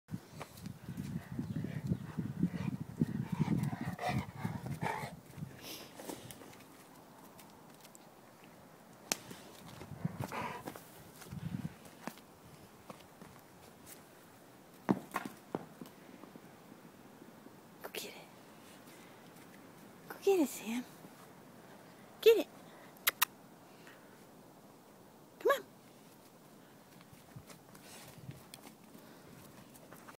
Uncle Sam Bullmastiff (available for adoption)
Uncle Sam is a 5 year old Bullmastiff, available for adoption in Connecticut